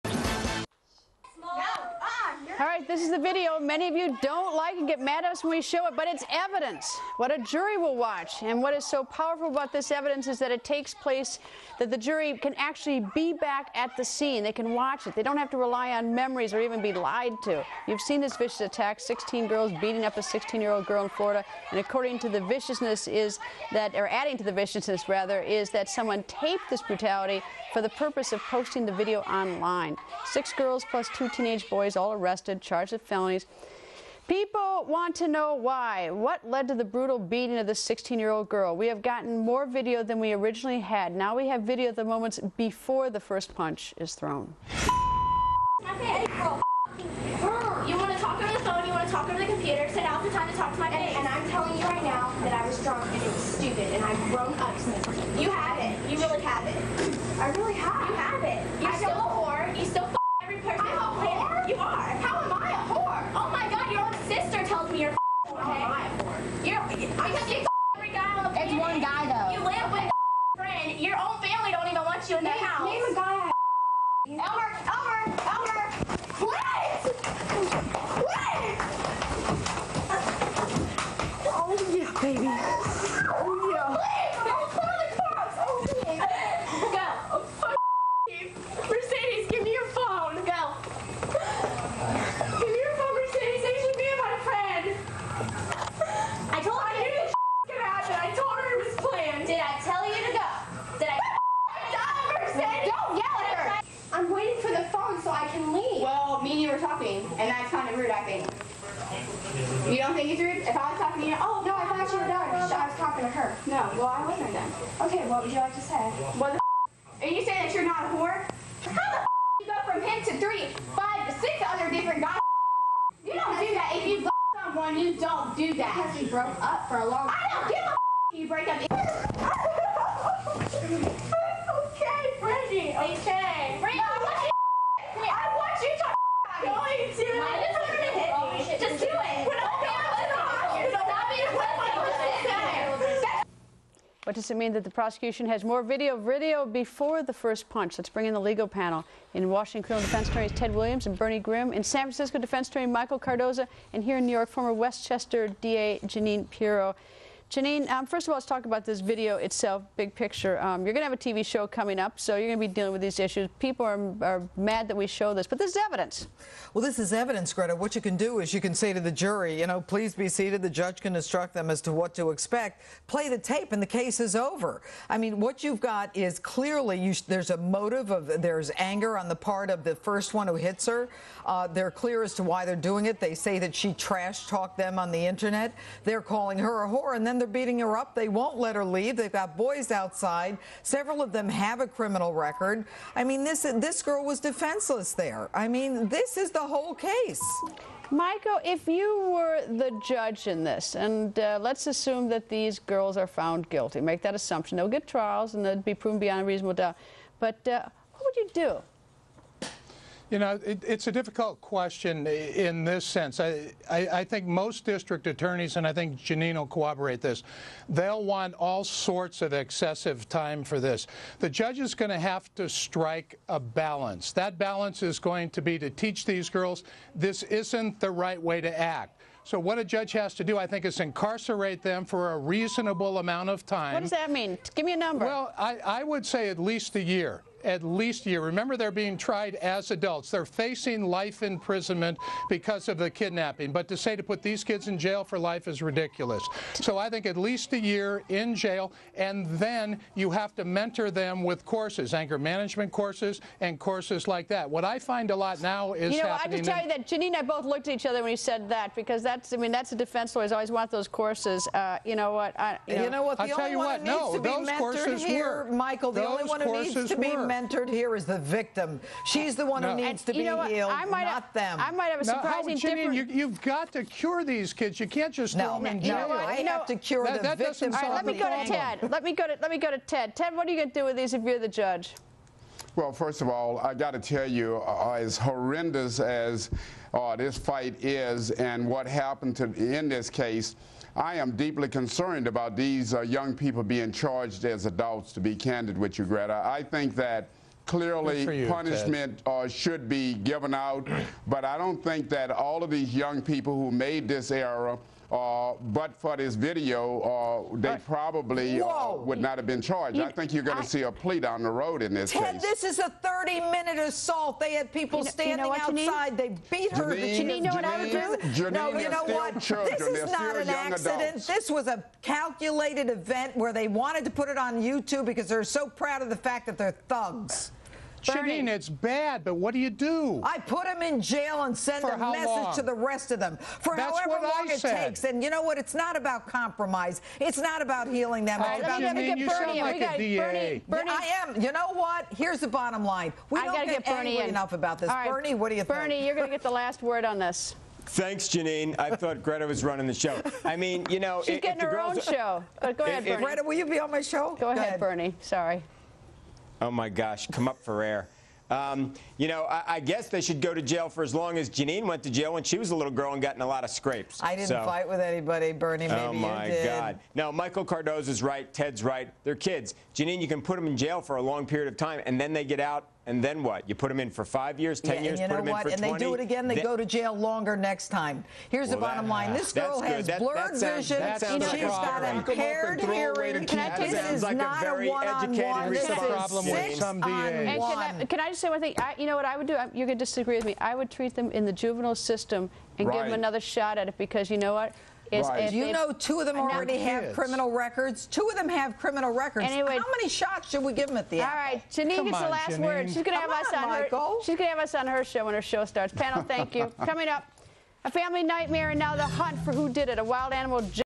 Lakeland Florida Teen Beating - Greta Van Susteren
Greta shows new footage of the first punch being thrown.